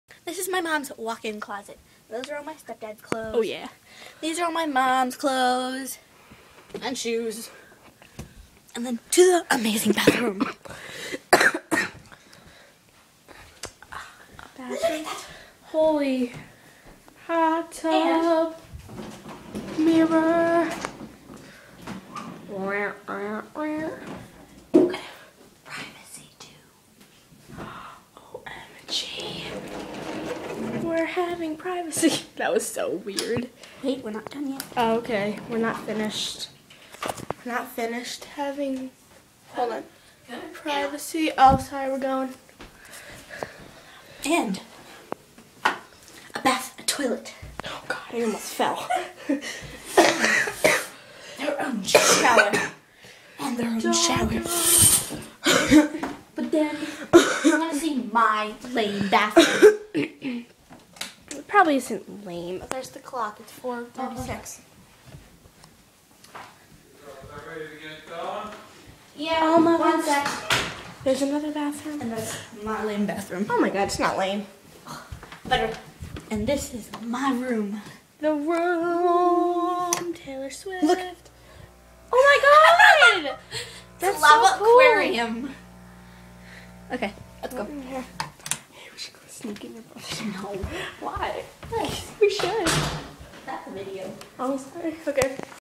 Sam's House!
Ok so this is Sam showing us around her awesome house.